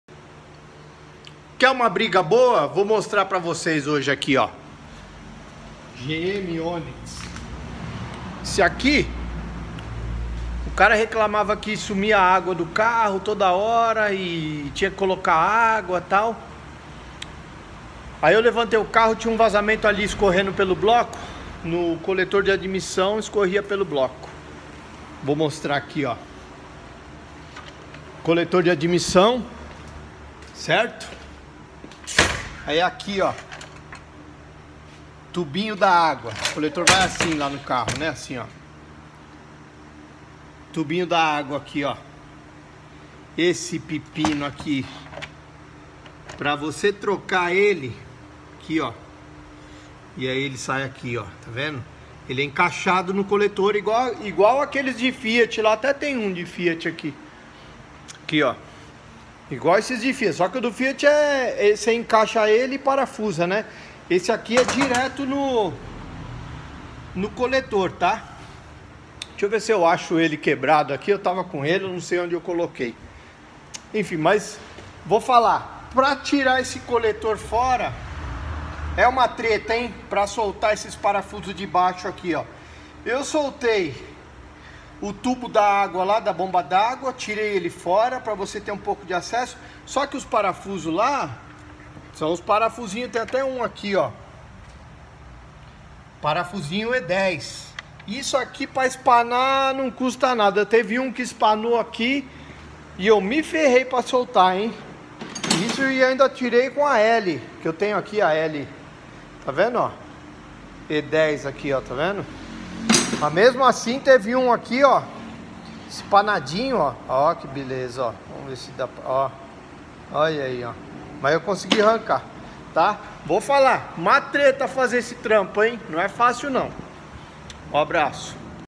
GM MOTOR SPE VAZAMENTO DE AGUA NO COLETOR ADMISSÃO
A treta de trocar o tubo de agua do coletor de admissão